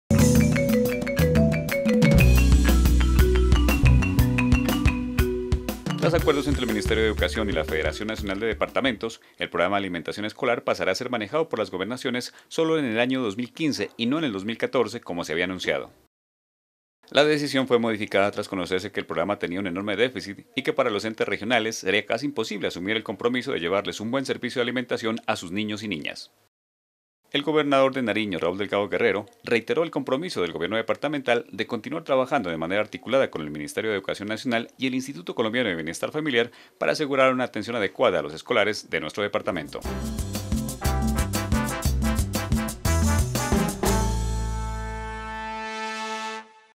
Programa de Alimentación Escolar para 2015 - dic 19
Luego de confirmarse el déficit que tiene el PAE se concertó entre departamentos y gobierno nacional que este programa no será administrado por los departamentos en el 2014 sino en el 2015.